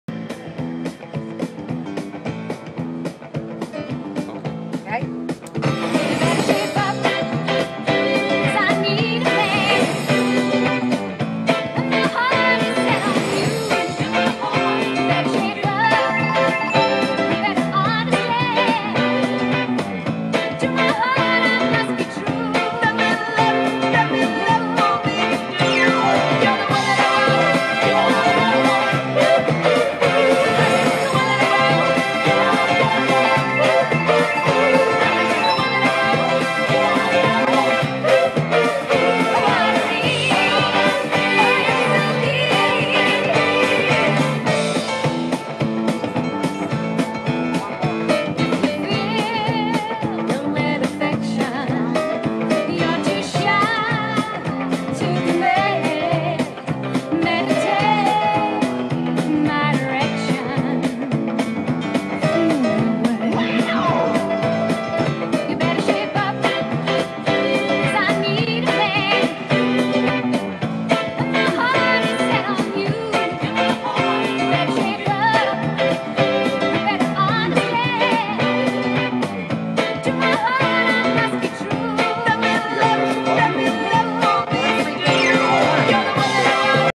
Magician in atlantic city NJ
A break from performing a magic show in New Jeraey